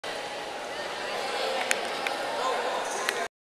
Barinas vs Merida , Campeonato nacional mini
LIGA NACIONAL MINIBASQUET . Maracaibo 2014.
Barinas 51 - Merida 46 , por Barinas Freddy Murillo 15 ptos , Ramses Perales 10 y David Castillo 08 ptos . Centro Gallego 02/10/2014